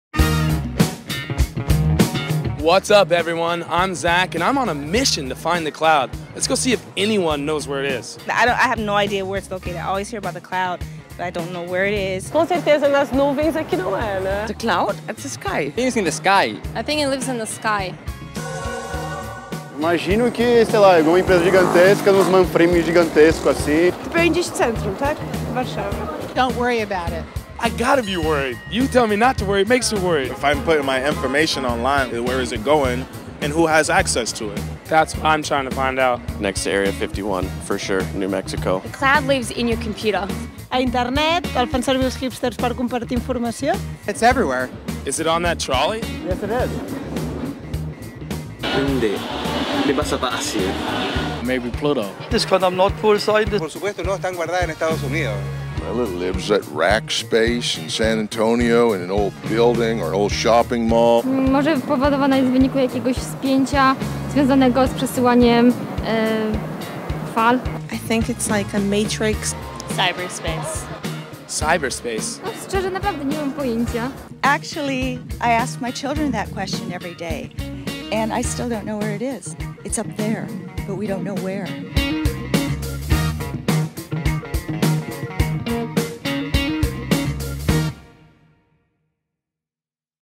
whereisthecloud episode 1
Most people use the public cloud, but do you know where it is? We traveled the world to find out.